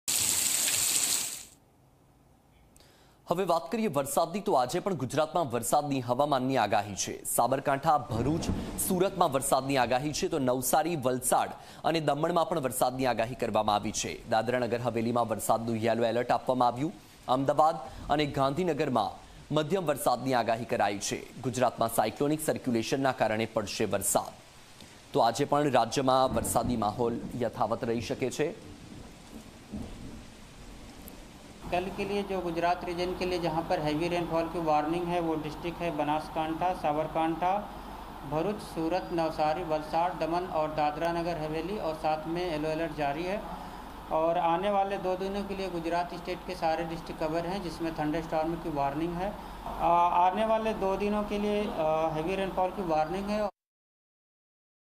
Gujarat Weather Forecast : Ahmedabad and Gandhinagar to receive moderate rain today | Monsoon 2024
આજે પણ ગુજરાતમાં વરસાદની હવામાનની આગાહી  --

સાબરકાંઠા, ભરૂચ, સુરતમાં વરસાદની આગાહી   --

નવસારી, વલસાડ અને દમણમાં વરસાદની આગાહી  --

દાદરા નગર હવેલીમાં વરસાદનું યલો અલર્ટ  --

અમદાવાદ અને ગાંધીનગરમાં મધ્યમ વરસાદની આગાહી  --

ગુજરાતમાં સાયક્લોનિક સર્ક્યુલેશનના કારણે પડશે વરસાદ  ---